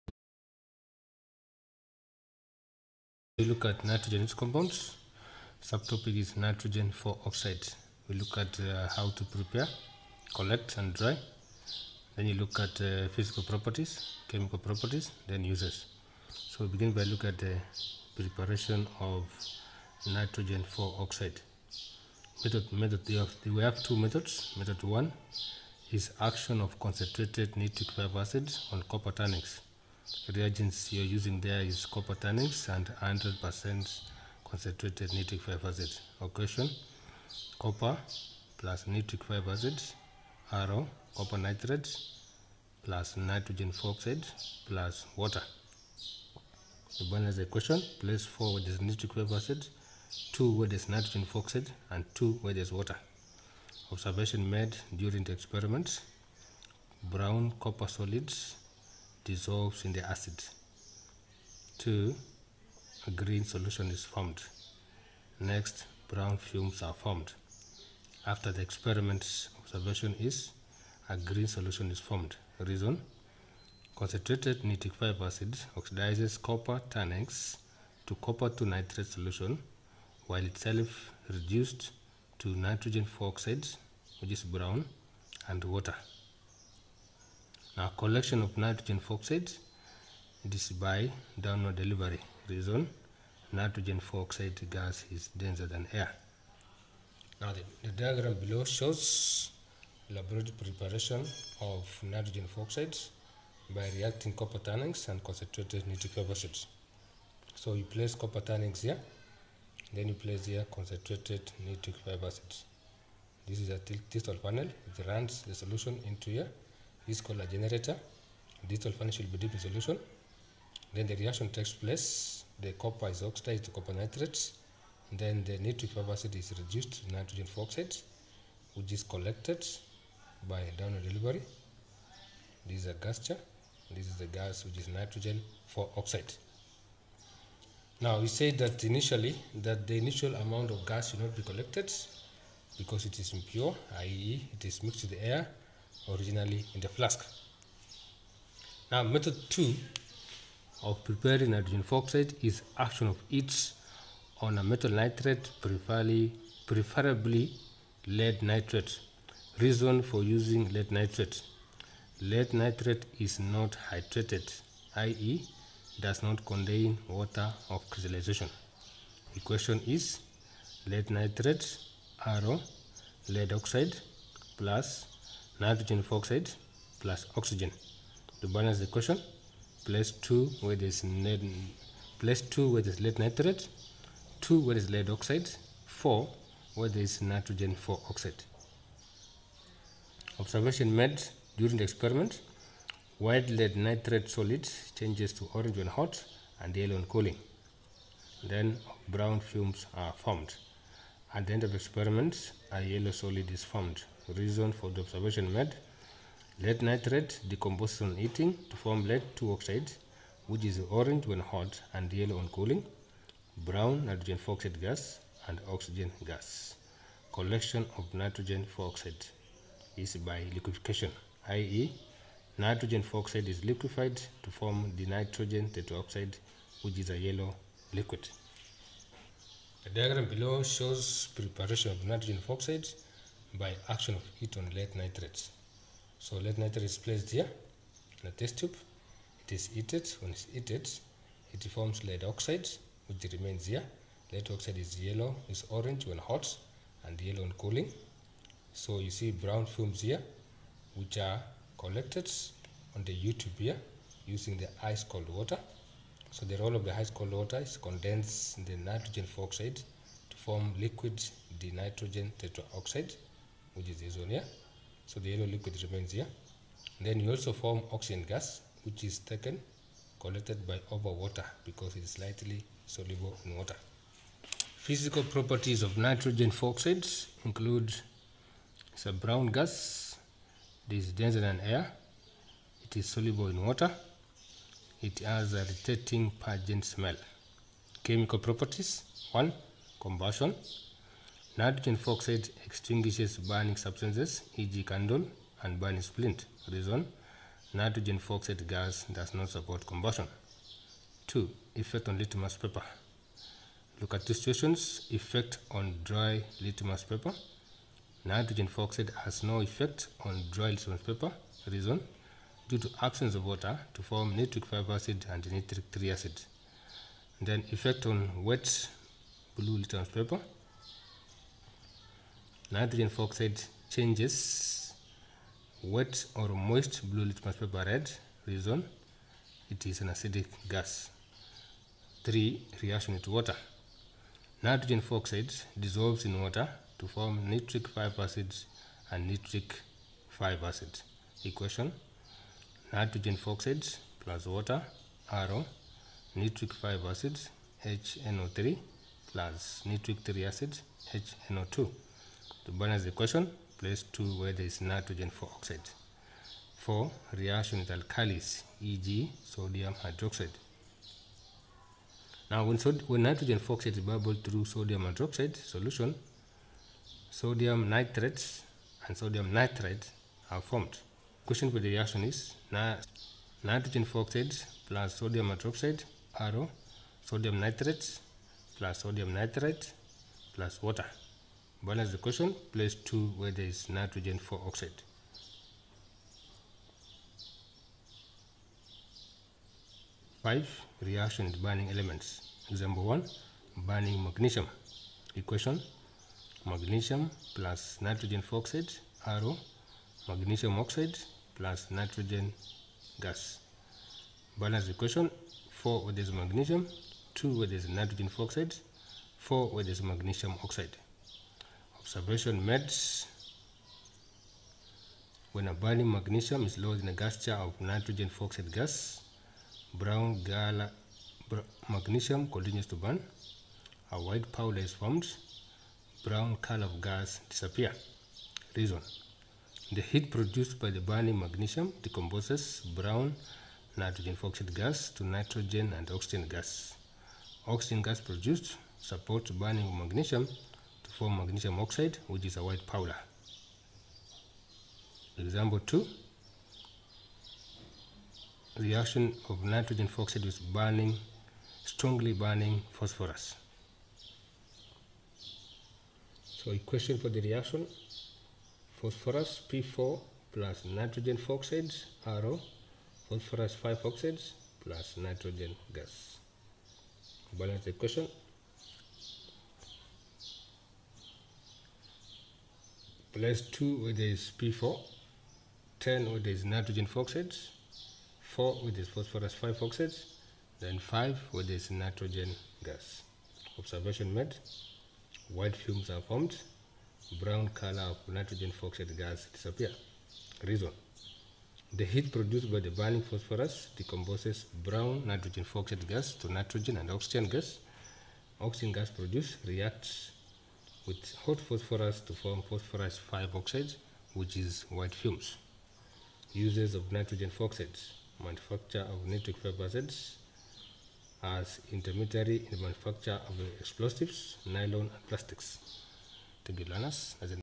FORM 3: NITROGEN AND ITS COMPOUNDS ( Preparation and Chemical properties of Nitrogen(IV)oxide gas)
SAMPLE K. C. S. E QUESTIONS
1. When nitrogen(IV)oxide gas is bubbled through potassium hydroxide two salts are formed
     a. Name the two salts
     b.  Give the reason for the formation of the two salts
     c.  Write a balanced chemical equation for the reaction that took     place
2. Nitrogen(IV)oxide is prepared in the laboratory by action of heat on Lead(II)nitrate
   a. Give a reason why Lead(II)nitrate is preferred
   b. Name another gas that is formed during the reaction
   c. State two observation made during the experiment
   d.  Nitrogen(IV)oxide gas is collected as a yellow liquid
           I. Name the method of collection and give a reason for your
              answer
           II. What is the name of the yellow liquid
   e. Write a balanced chemical Equation for the reaction that took place
3.  State and explain all observations made when a burning magnesium is lowered in the gas jar containing Nitrogen(IV)oxide gas
4. State and explain the observation made when dry litmus paper is lowered in the gas containing wet Nitrogen(IV)oxide gas
5. When strongly burning Phosphorus is lowered in the gas jar containing Nitrogen(IV)oxide gas . White fumes are formed
   a. Name the white fumes
   b.  Write the Equation for the reaction that took place
   c. state and explain any other observation made during the reaction
5. When Concentrated Nitric(V)acid is added into copper turnings , a green solution Z and brown gas A are formed
   a.  Name solution Z and gas A
   b. What property of Concentrated Nitric(V) acid is illustrated in the experiment
   c.  Write the chemical Equation for the reaction that took place

NEXT LESSON
• Laboratory preparation of ammonia gas
• industrial manufacture of ammonia gas
• chemical properties of ammonia